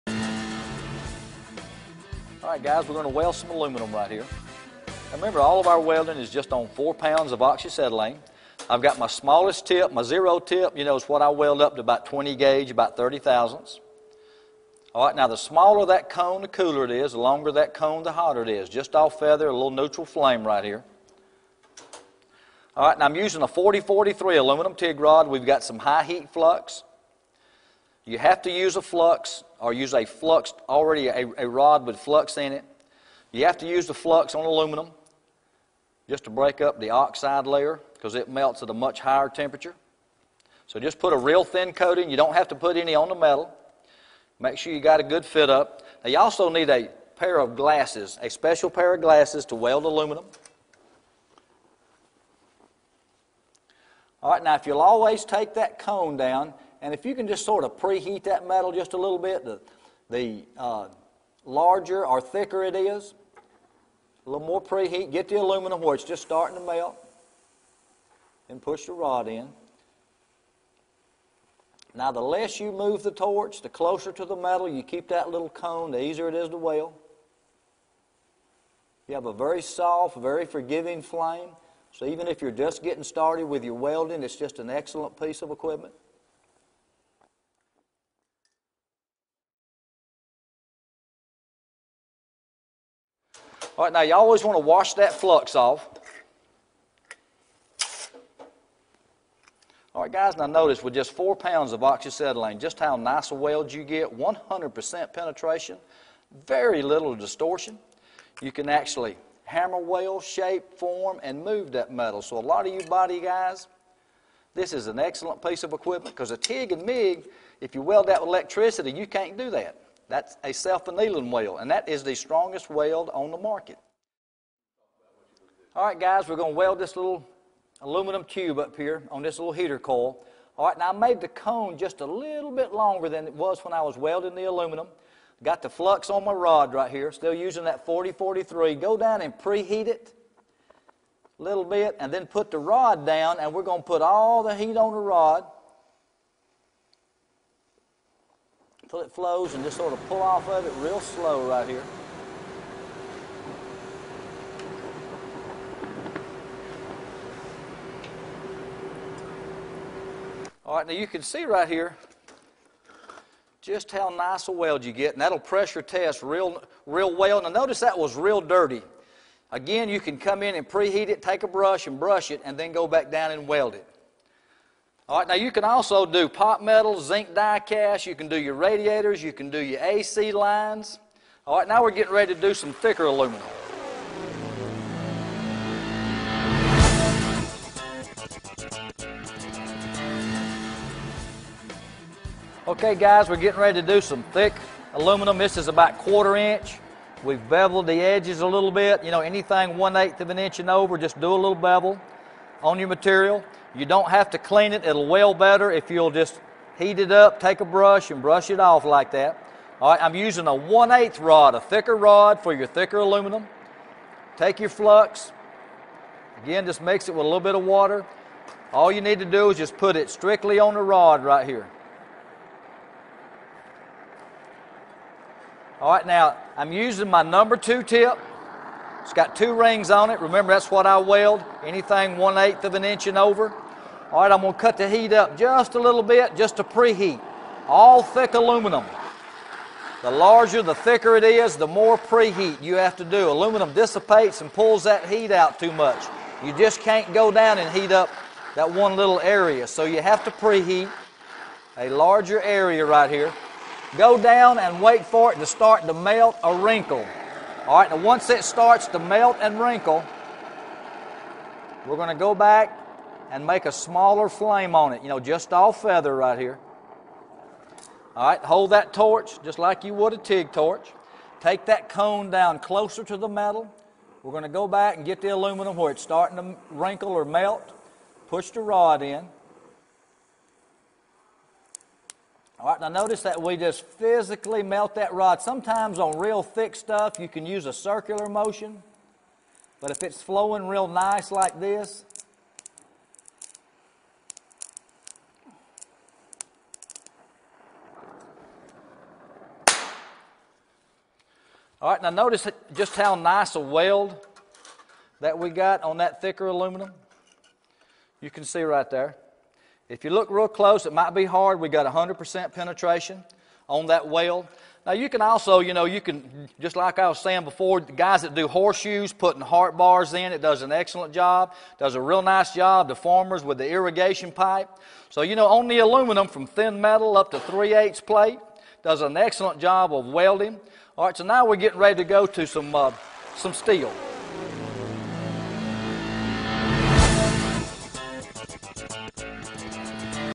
CH 10 DHC2000 Instructional DVD Welding Thin Aluminum
2020 Revised Instructional DVD for the DHC2000 oxygen and acetylene welding and cutting torch.